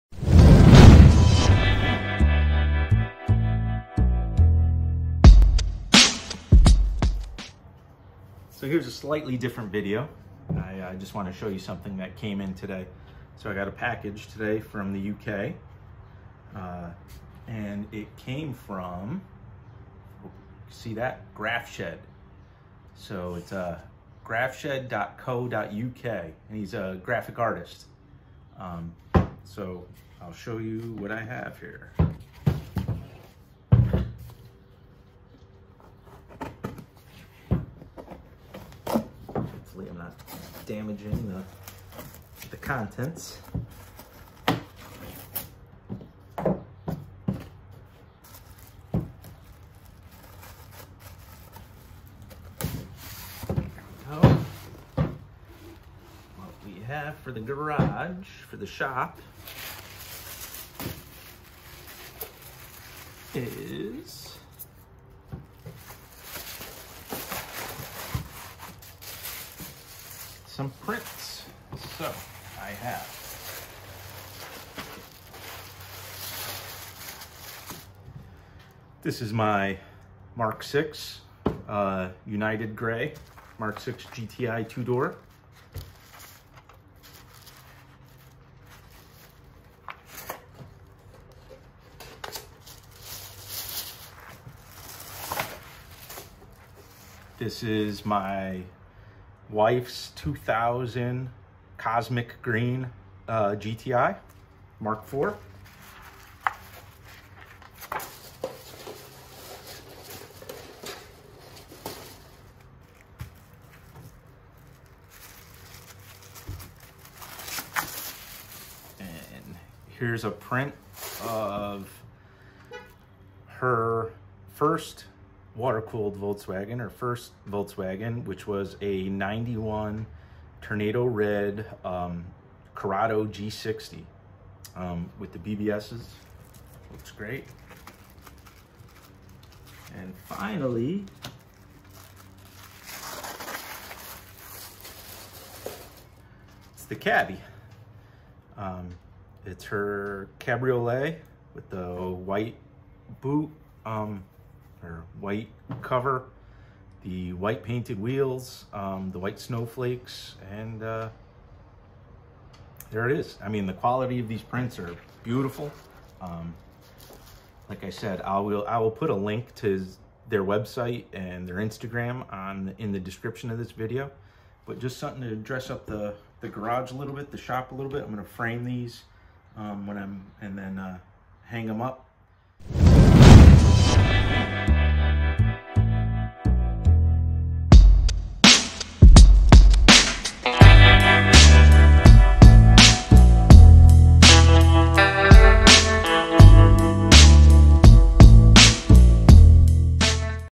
Ordered Artwork for the Garage: Review of Grafshed
I ordered a few custom prints of some of the VWs we have owned. I ordered them from Grafshed in the UK and am very happy with the outcome.